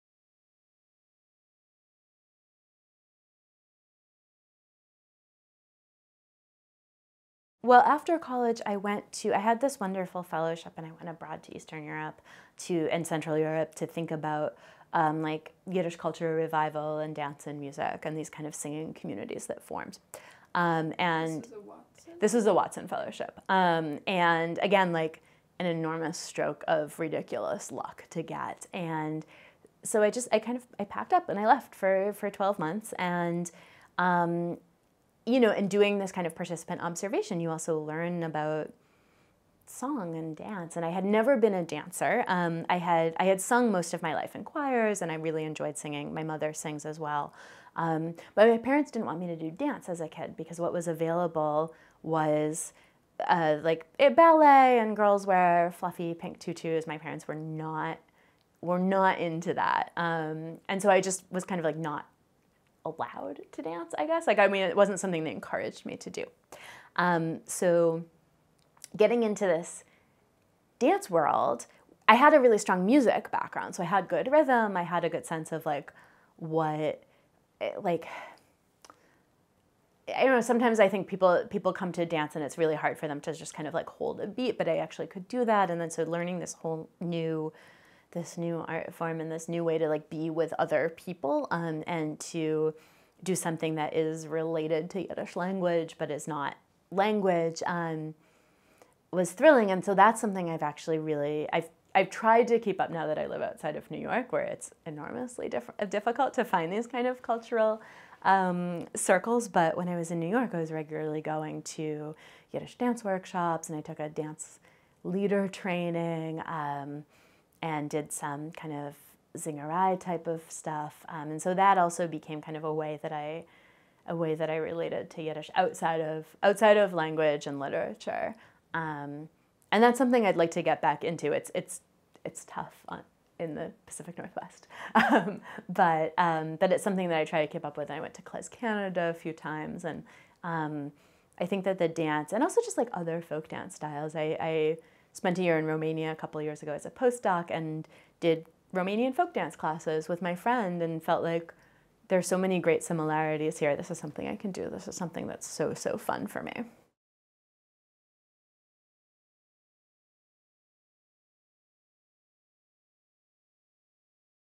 "I Packed Up My Things and Left": Studying Song and Dance in Eastern Europe
Sarah Ellen Zarrow, scholar of Jewish history, describes her experience doing ethnographic research in Eastern Europe.

To see the full interview and learn more about the Yiddish Book Center’s Wexler Oral History Project,